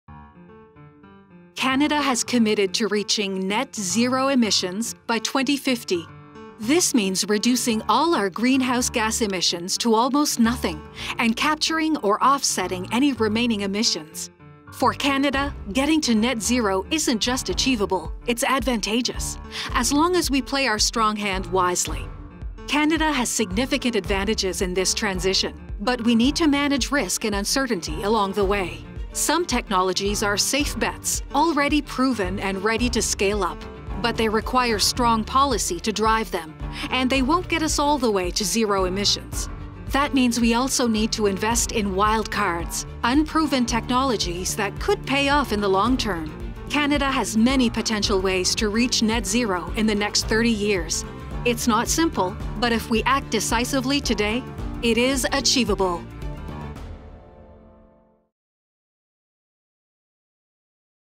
Reaching Net Zero in Canada is achievable, here's why.
The Canadian Institute for Climate Choices (CICC) published its first in-depth scenario report to explore how Canada can reach net zero emissions by 2050. It shows that to achieve Net Zero, Canada must rely on stronger policy and strategic bets.